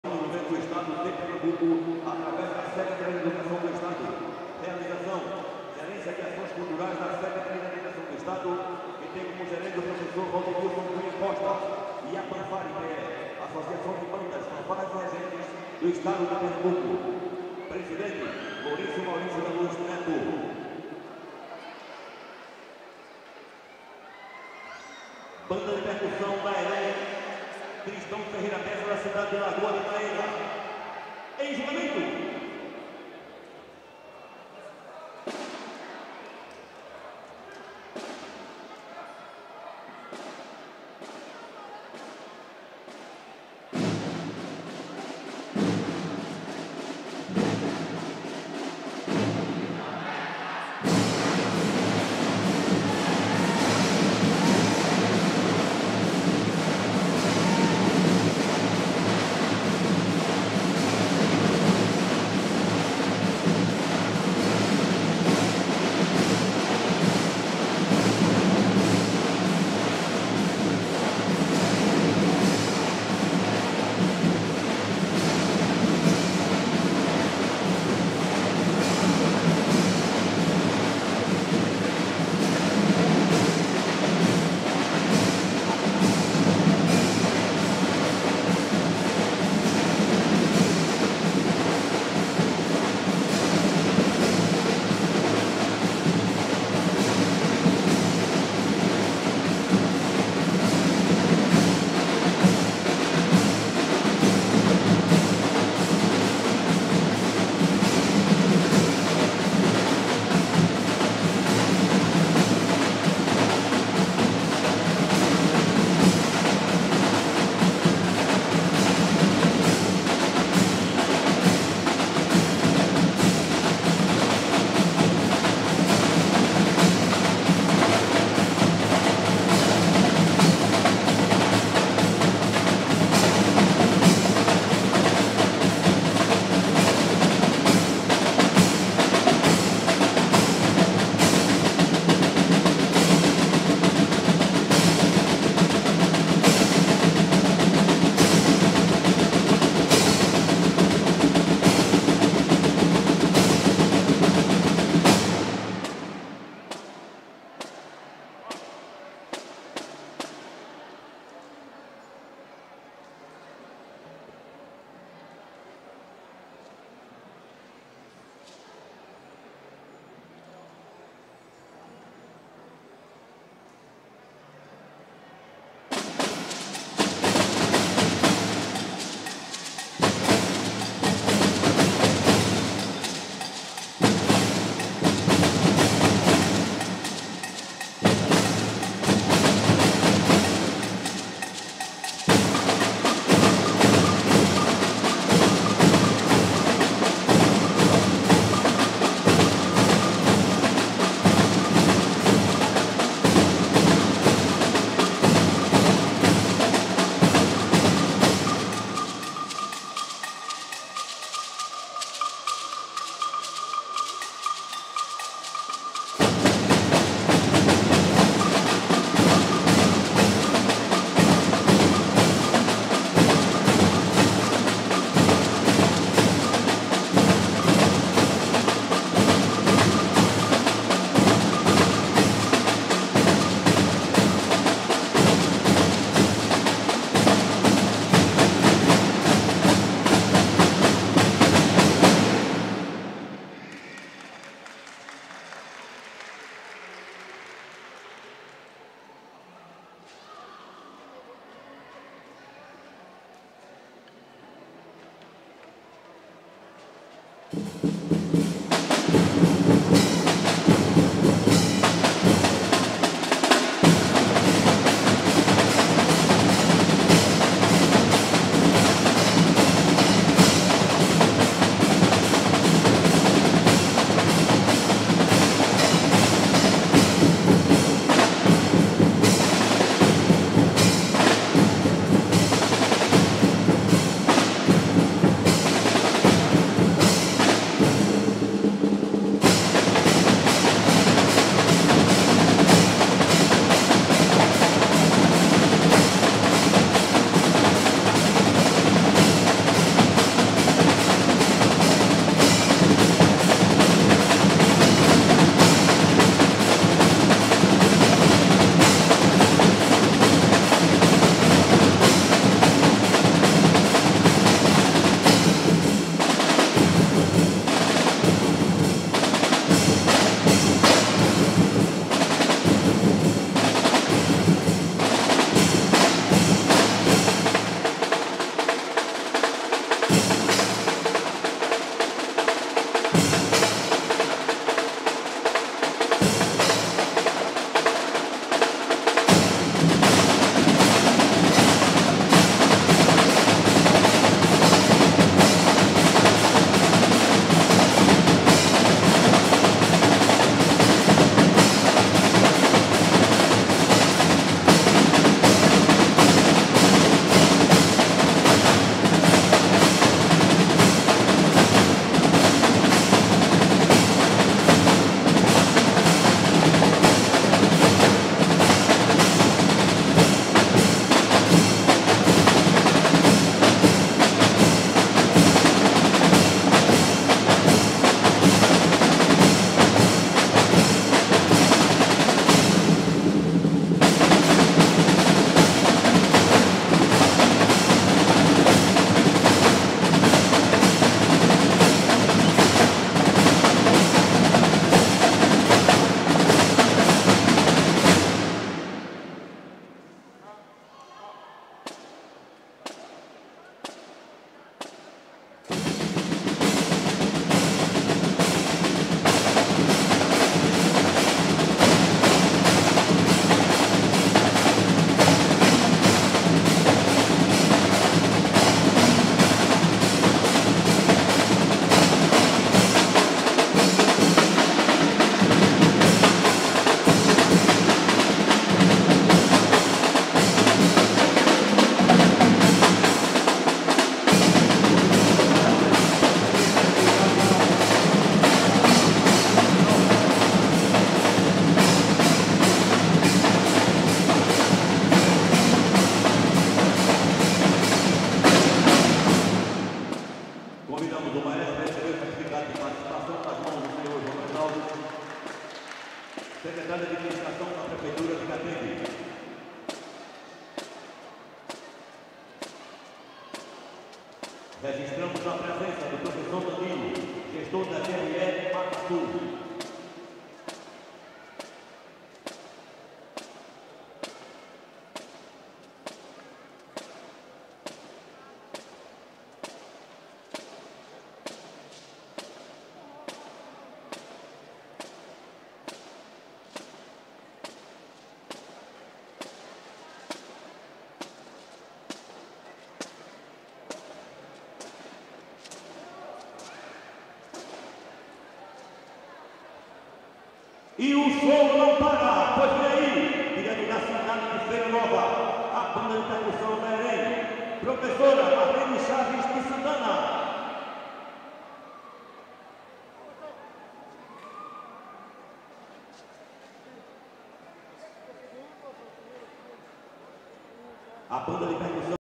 EREM TRISTÃO FERREIRA BESSA - PERCUSSÃO RUDIMENTAR
IV Etapa da XIII Copa Pernambucana de Bandas e Fanfarras, Catende.